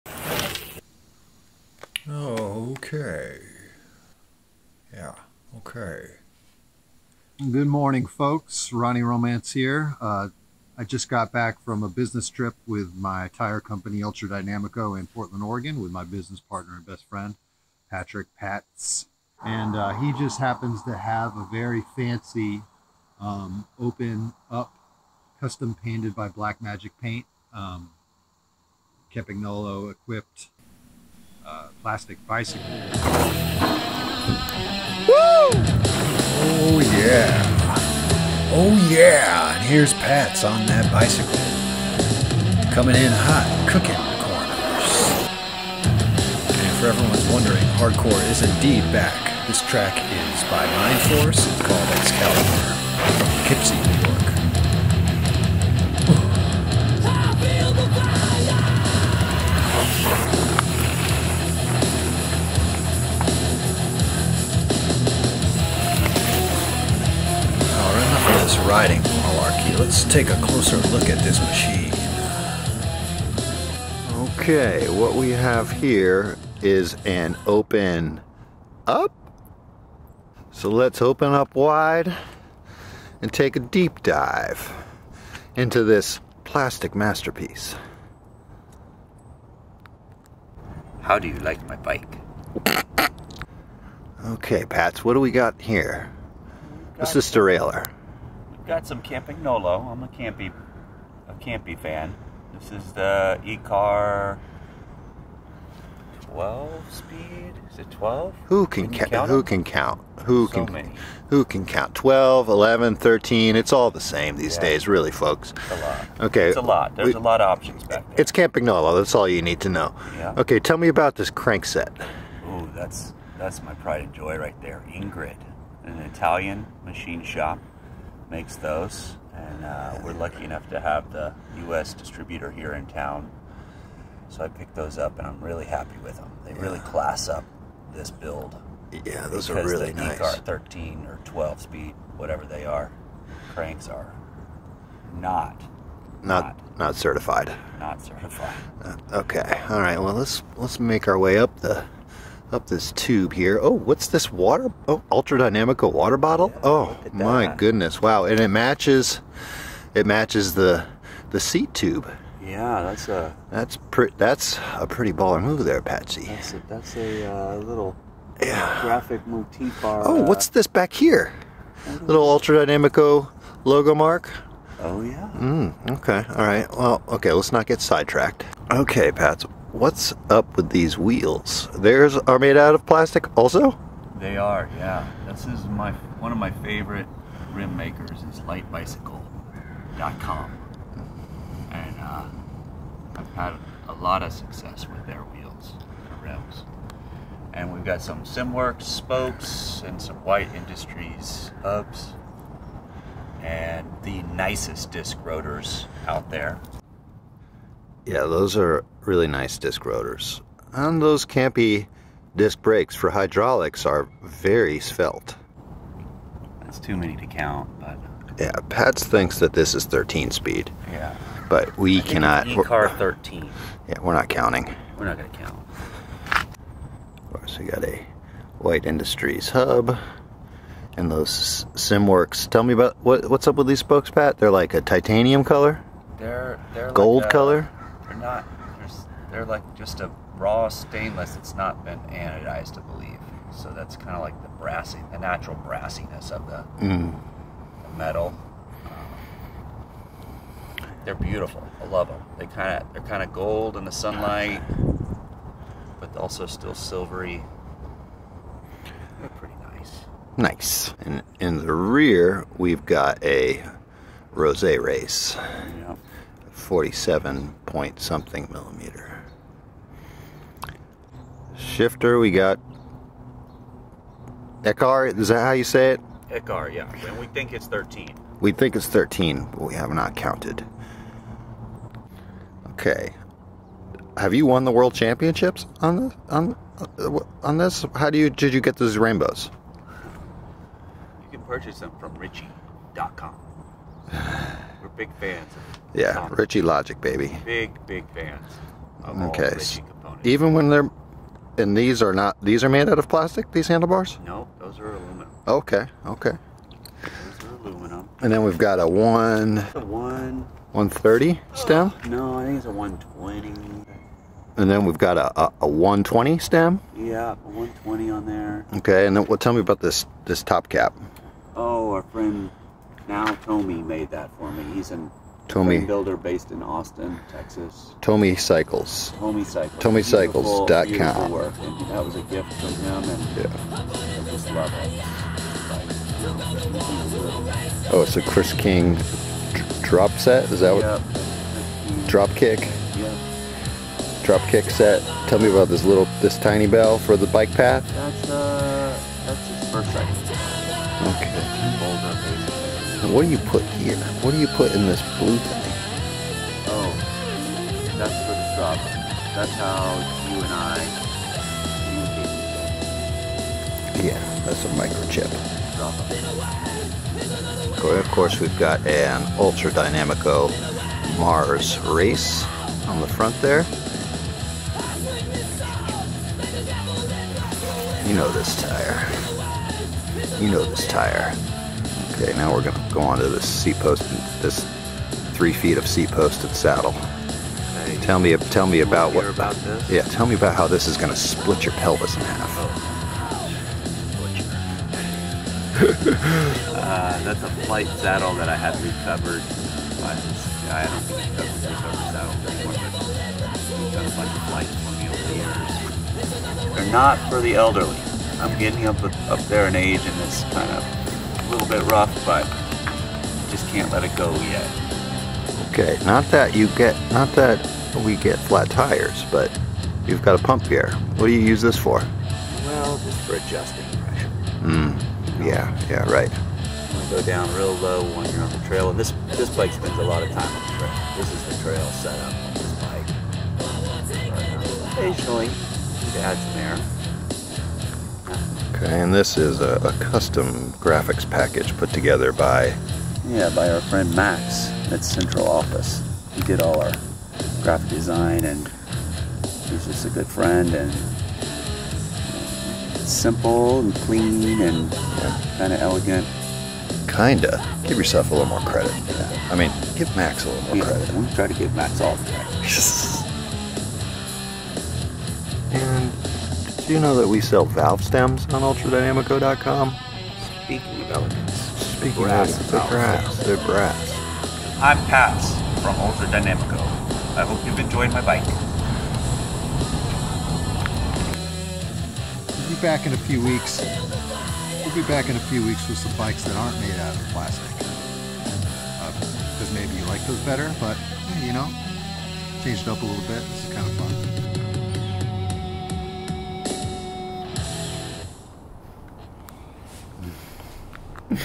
Patz of Ultradynamico's OPEN U.P. space bike
something of a different material and locationl for you this week ... a fashionable and tastefully done grav dad bike curated by my best friend and biz partner PATZ.